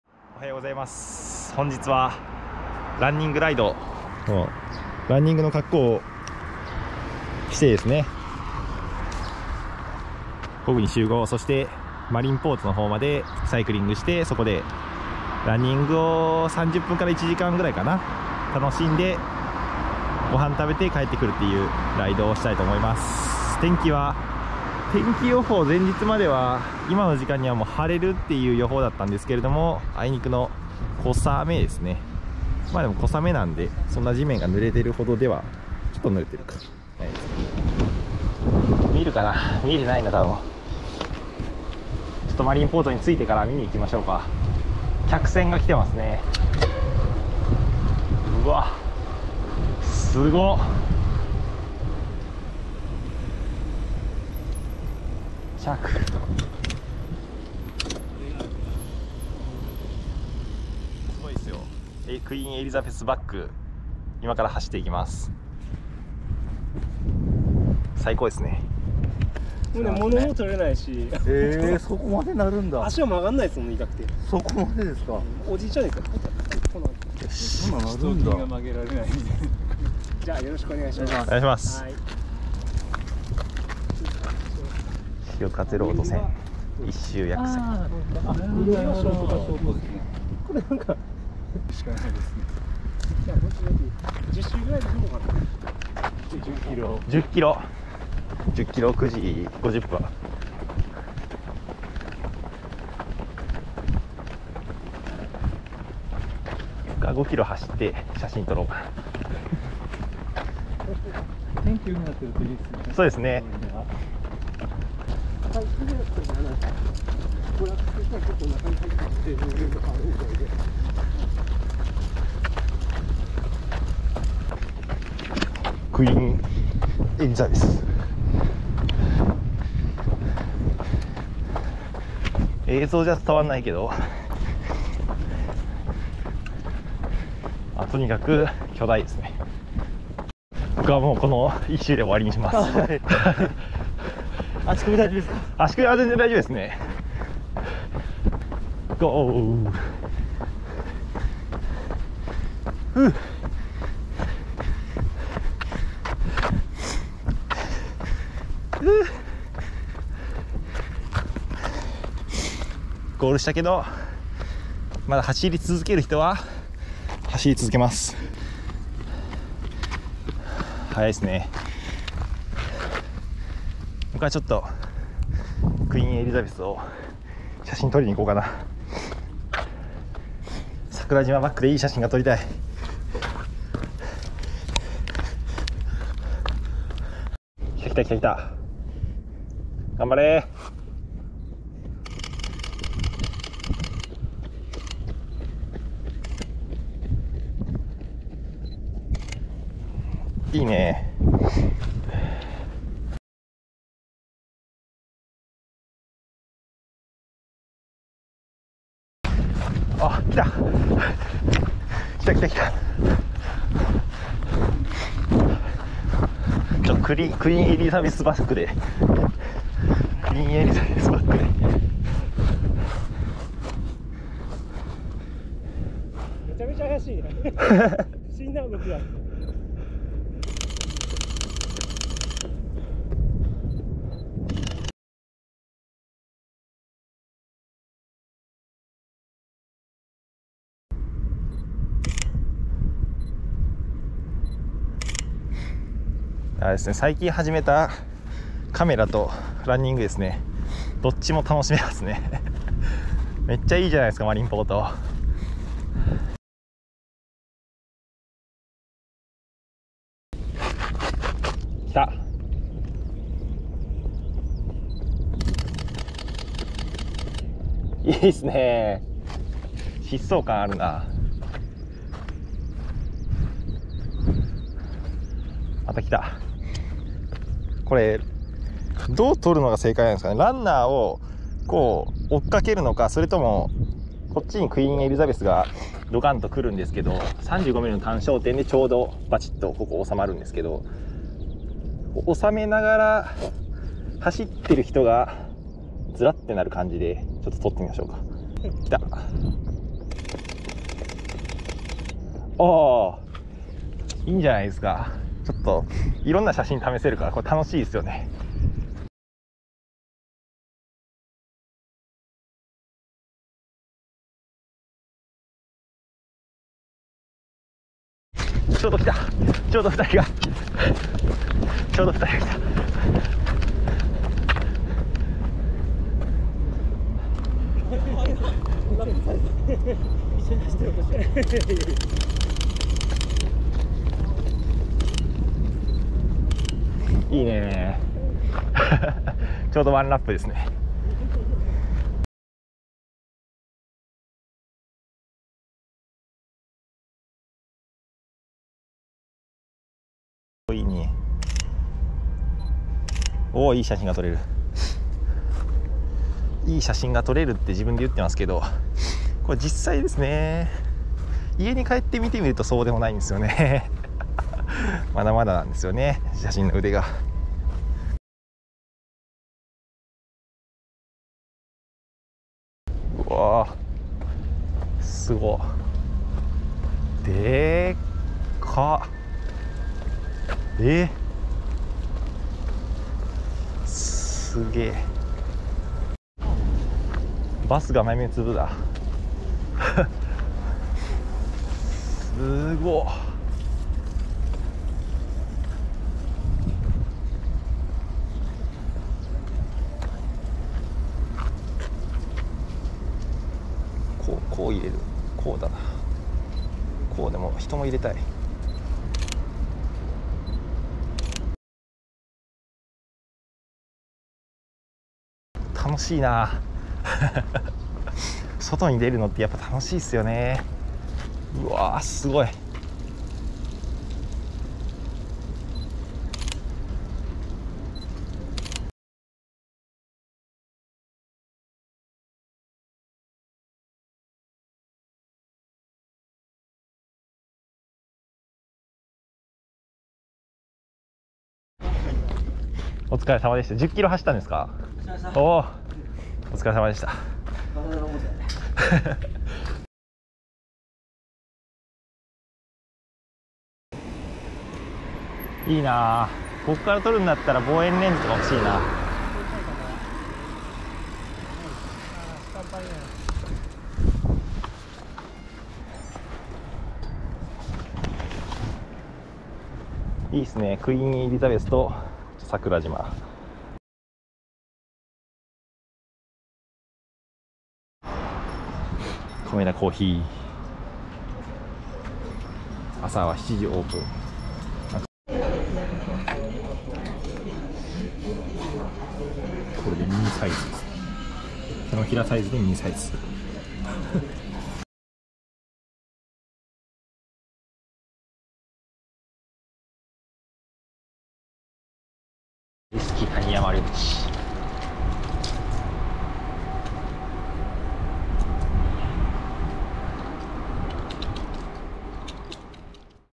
マリンポートにてランニングライド！【ロードバイク】
マリンポートまでは自転車で行き、みんなで楽しくランニング！
クイーンエリザベス号が停泊していました。カメラを持って行って良かった！

GoPro HERO9で撮影。